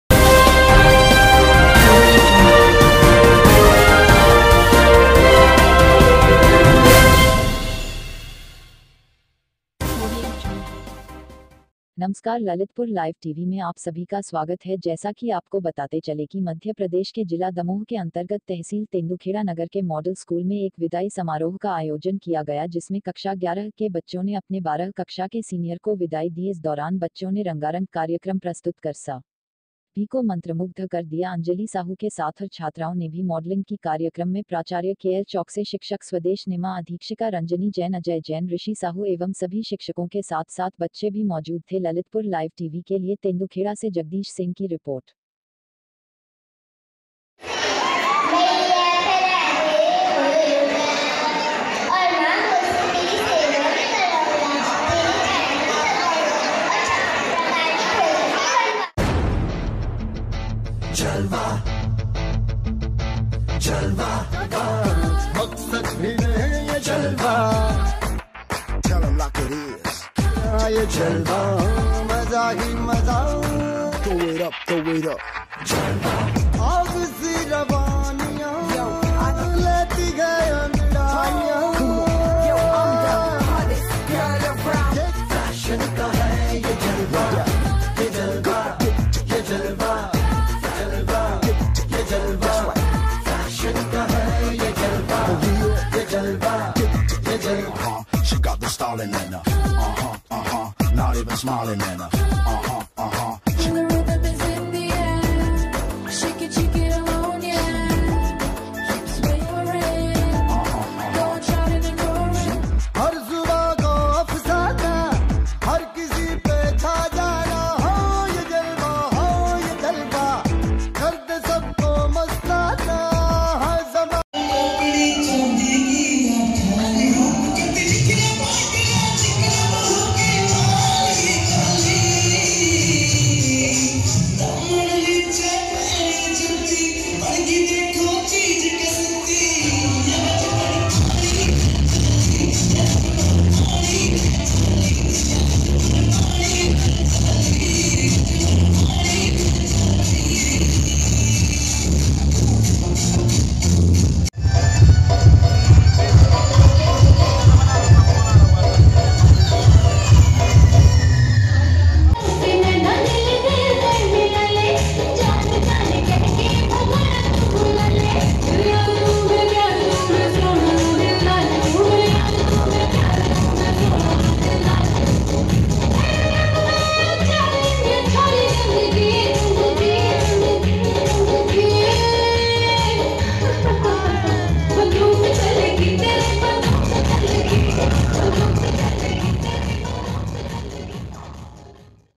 दमोह - तेंदूखेड़ा नगर के मॉडर्न स्कूल में विदाई कार्यक्रम के दौरान स्कूली छात्र छात्राओं ने किया
एक से एक बढ़कर रंगारंग कार्यक्रम देखिए ललितपुर लाइव टीवी की रिपोर्ट में